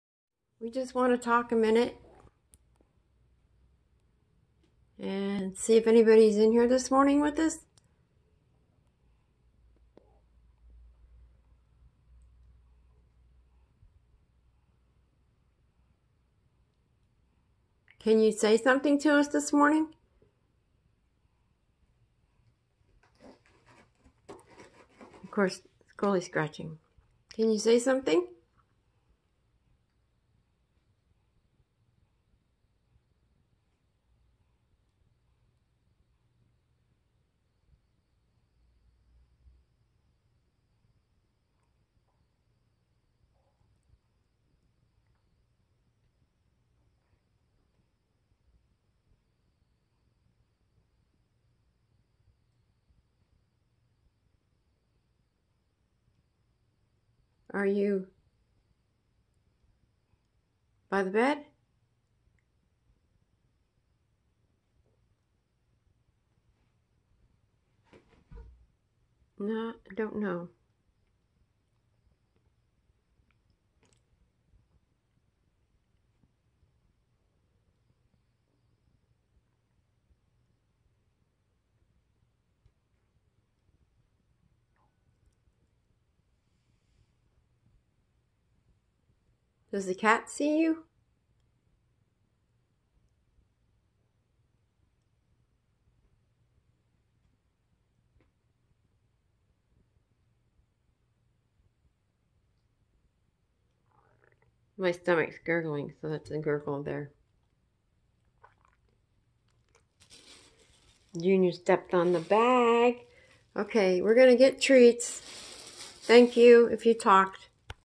cats and I get verbal spirit responses. most clear is a "no".
just checked to see if we were with company, can't figure out first response, but then hear a clear "no" answer later.